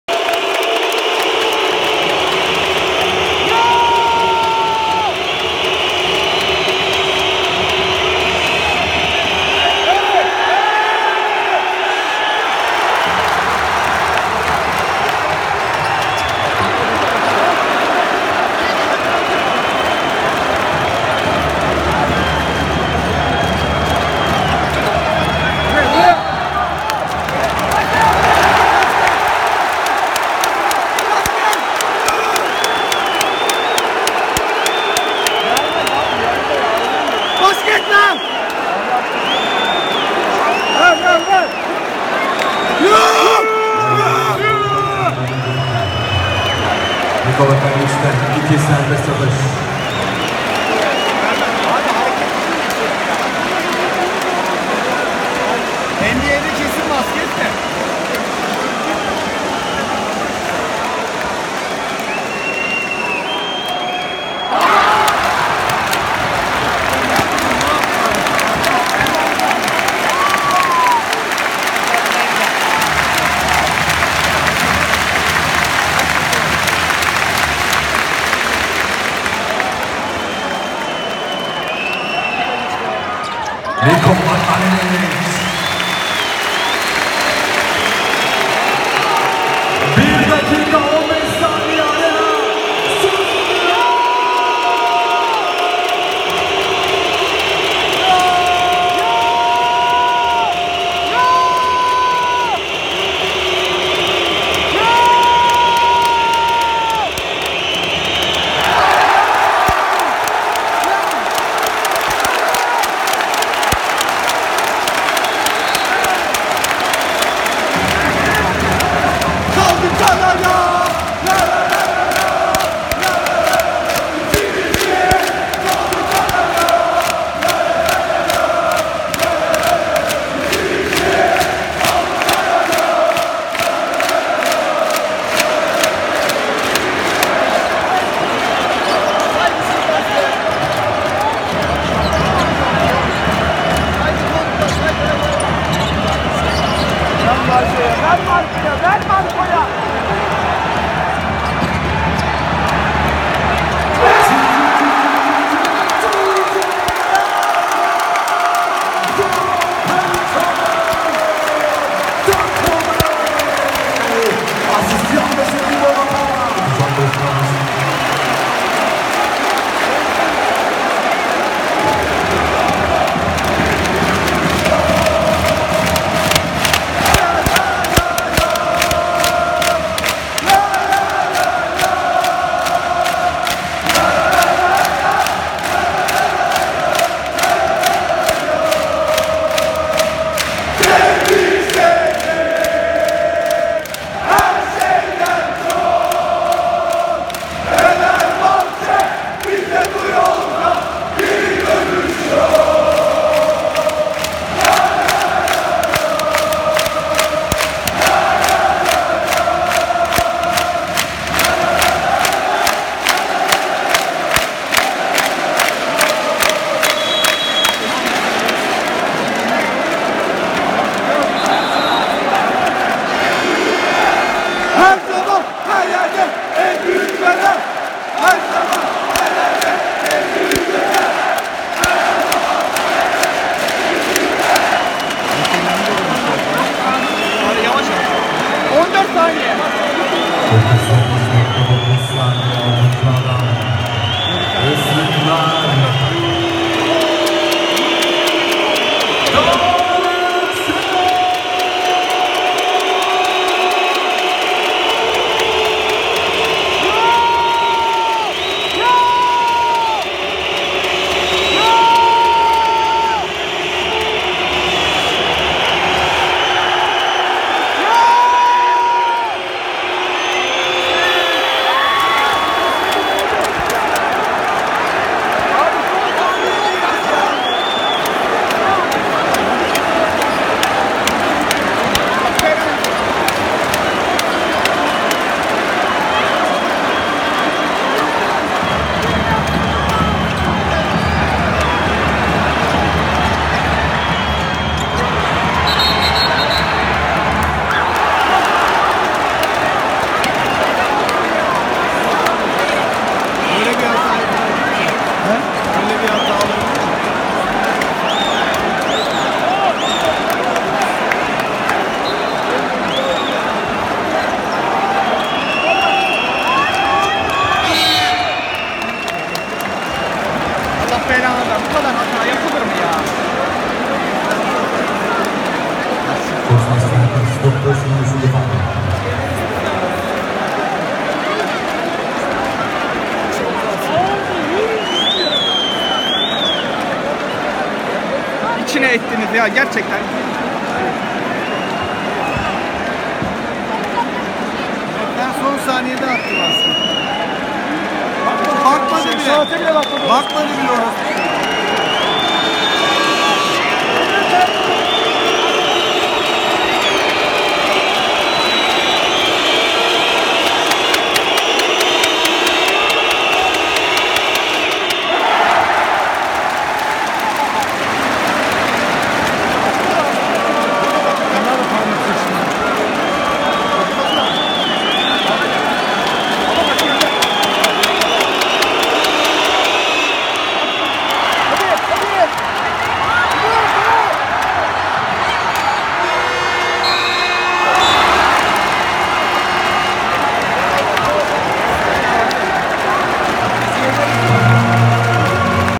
Fenerbahçe Beko 80-82 Zalgiris Kaunas | Taraftarın Takımı İttiği Maçın Son Anları
Fenerbahçe Beko 80-82 Zalgiris Kaunas Turkish Airlines Euroleague Playoff 2. Maç  | Fenerbahçe Ülker Sports Arena

THY Euroleague Ligi play-off çeyrek final ikinci maçında Fenerbahçe Beko, konuk ettiği Litvanya temsilcisi Zalgiris'e 82-80 yenildi ve seride durum 1-1 oldu.

THY Euroleague play-off çeyrek final ikinci maçında sahasında Zalgiris'e 82-80 yenilen Fenerbahçe Beko'nun Avrupa'da iç sahadaki 18 maçlık galibiyet serisi sona erdi.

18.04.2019